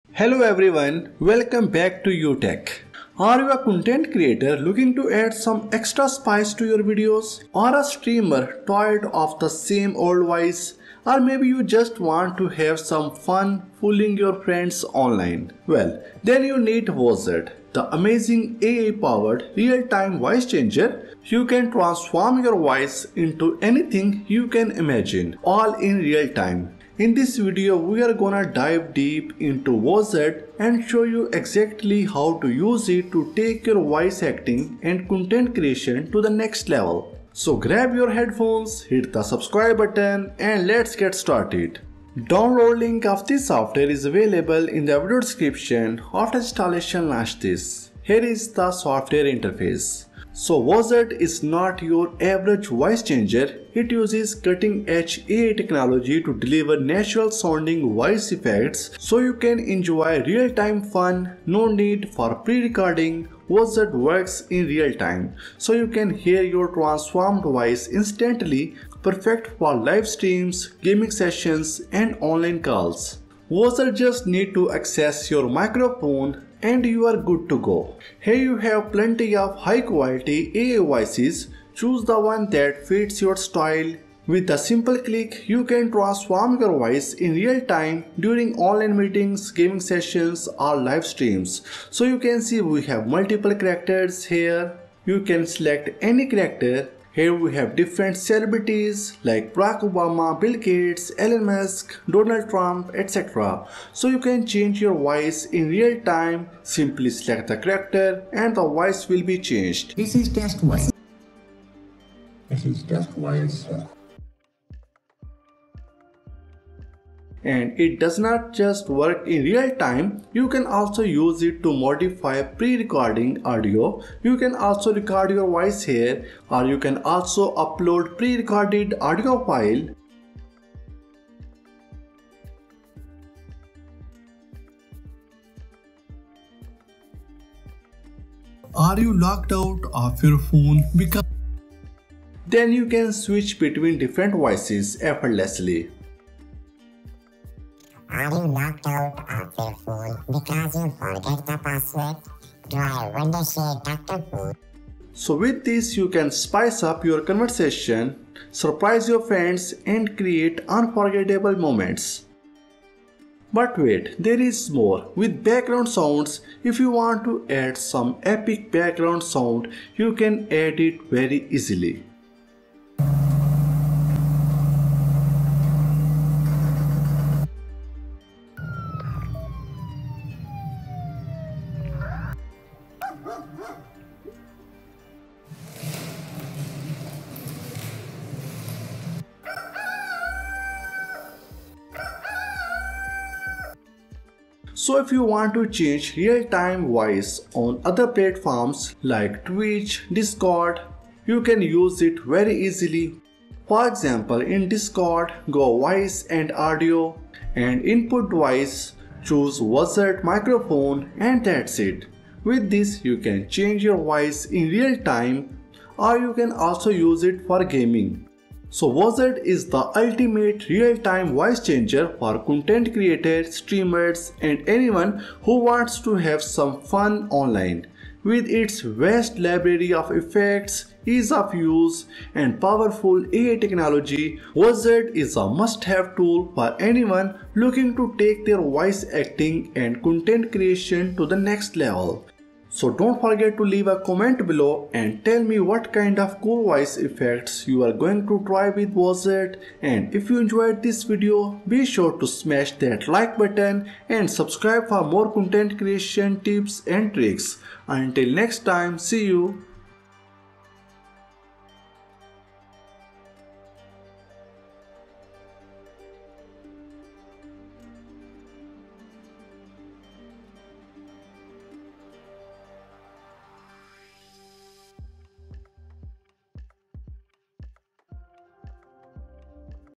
How to Change your Voice in Real-time in Discord
Want to sound like a robot, anime character, or something else entirely? This video shows you how to use a AI voice changer to modify your voice in real-time on Discord!  Easy setup, fun effects - get ready to surprise your friends!